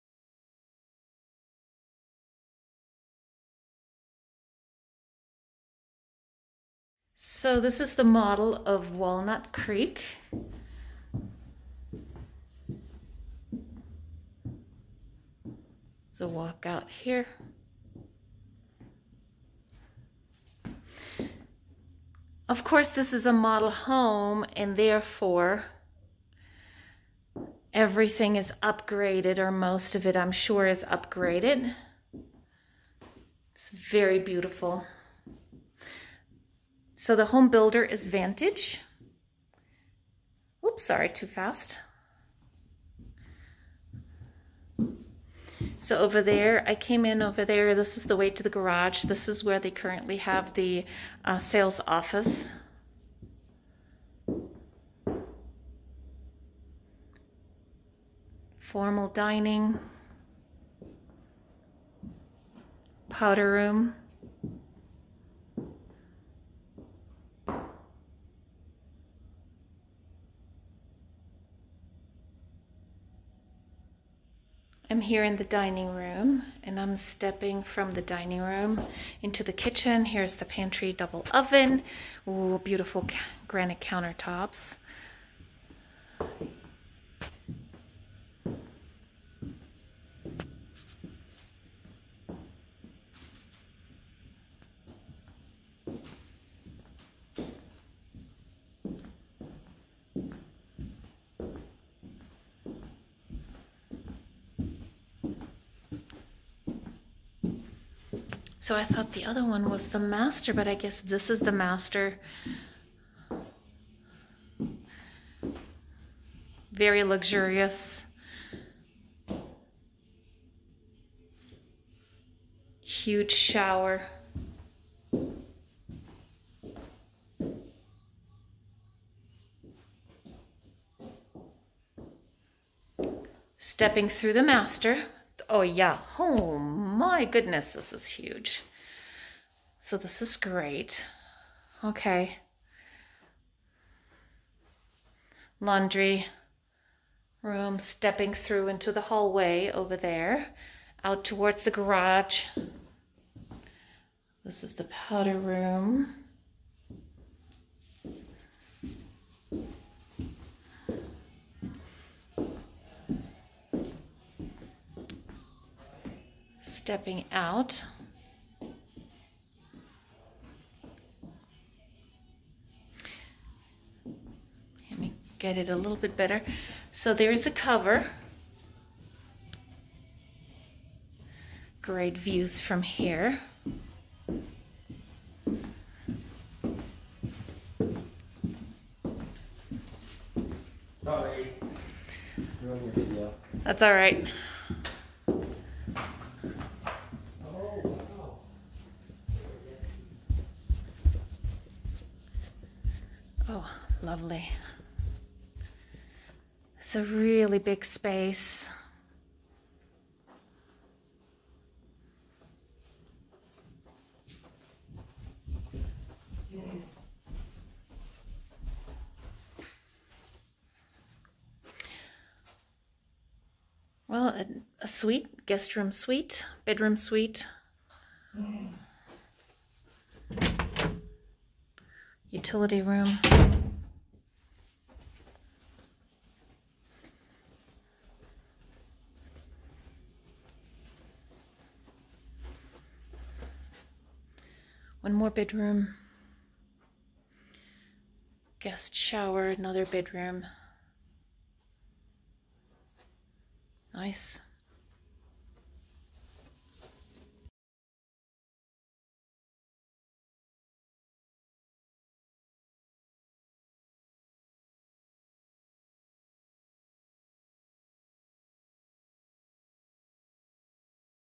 The Copperwood Model House
This is a walkthrough of Copperwood Model by Vantage Homes in Flying Horse.

Looking to move to Colorado Springs? We have a
✔️neighborhood guide
✔️school guide
✔️home buyer guide
✔️home sellers guide